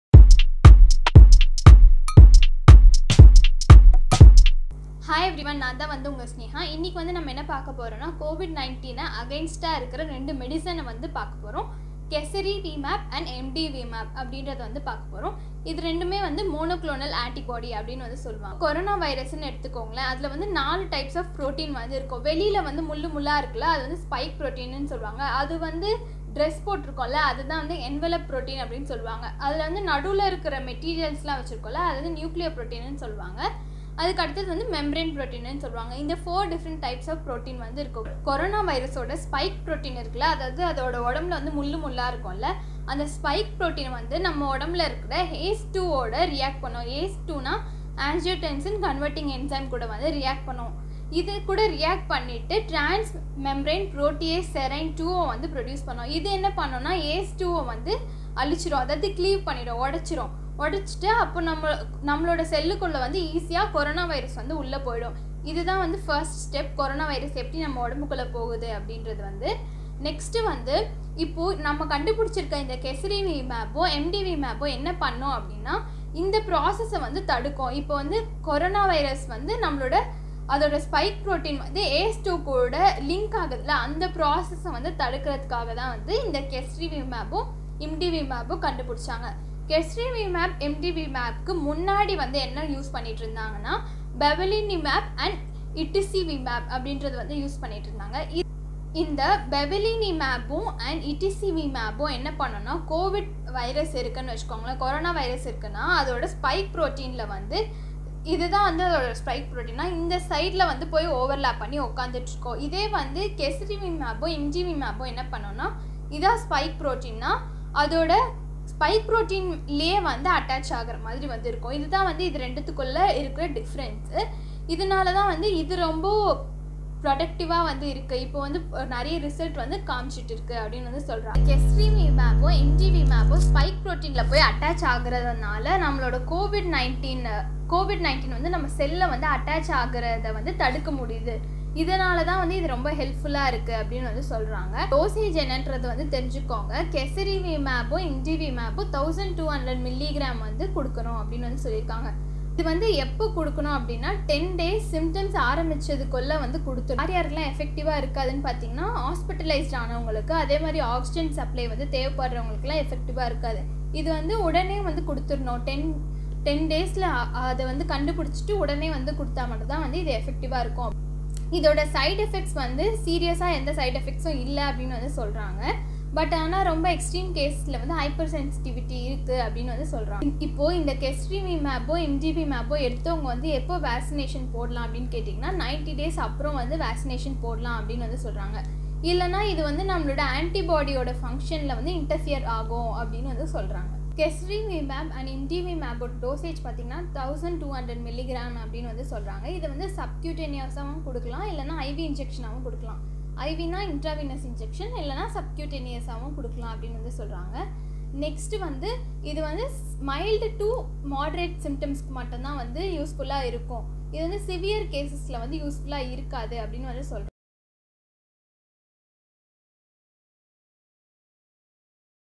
CASIRIVIMAB AND IMDEVIMAB FOR CORONA PATIENTS IN TAMIL
Casirivimab/imdevimab, sold under the brand name REGEN-COV, is an experimental medicine developed by the American biotechnology company Regeneron Pharmaceuticals. It is an artificial "antibody cocktail" designed to produce resistance to the SARS-CoV-2 coronavirus responsible for the COVID-19 pandemic. Wikipedia
Other names: REGN-COV2
License data: US DailyMed: Casirivimab
Imdevimab: Monoclonal antibody against spike protein of SARS-CoV-2
Casirivimab: Monoclonal antibody against spike protein of SARS-CoV-2

SNEKA T.J is about BASIC MEDICINE. We cover lots of stuff about medicine so that we reach all grounds of people.